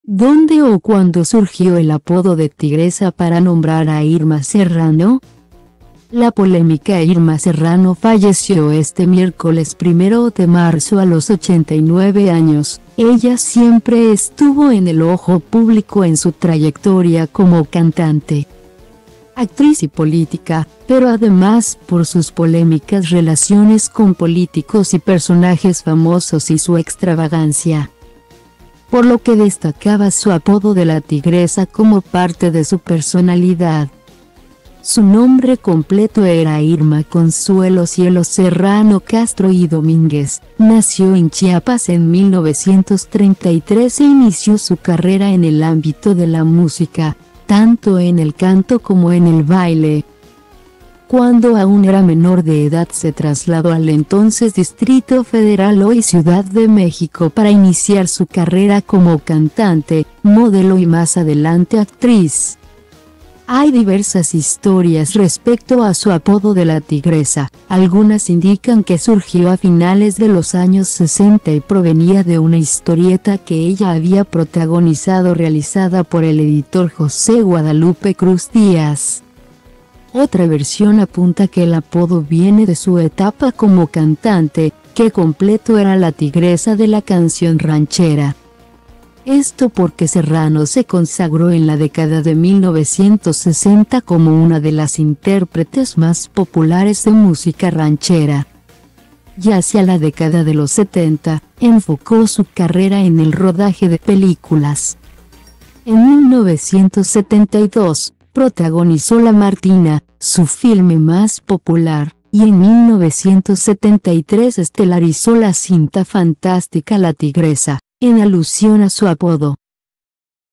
¿Dónde o cuándo surgió el apodo de Tigresa’ para nombrar a Irma Serrano?
La polémica Irma Serrano falleció este miércoles 1 de marzo a los 89 años, ella siempre estuvo en el ojo público en su trayectoria como cantante,actriz y política, pero además por sus polémicas relaciones con políticos y personajes famosos y su extravagancia,por lo que destacaba su apodo de “La Tigresa” como parte de su personalidad.